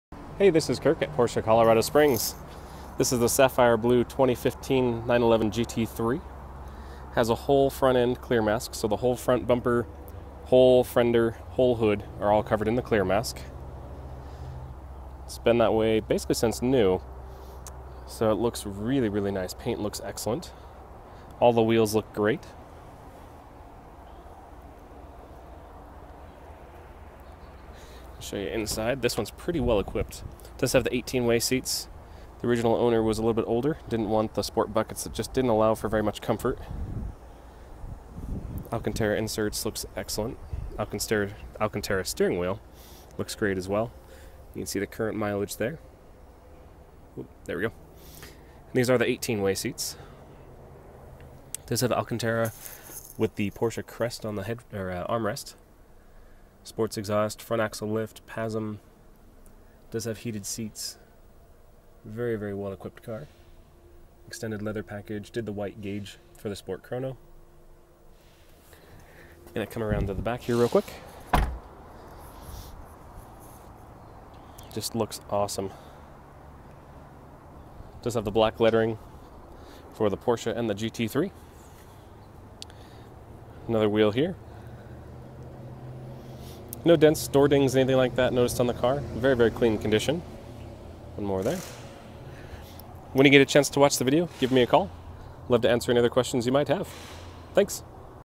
2015 Porsche CPO 911 GT3 with Kirk
Stunning Sapphire Blue Metallic GT3 with full hood clear mask and about $24,000 in options! Comes with an existing Porsche CPO warranty through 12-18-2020 and a clean carfax. C146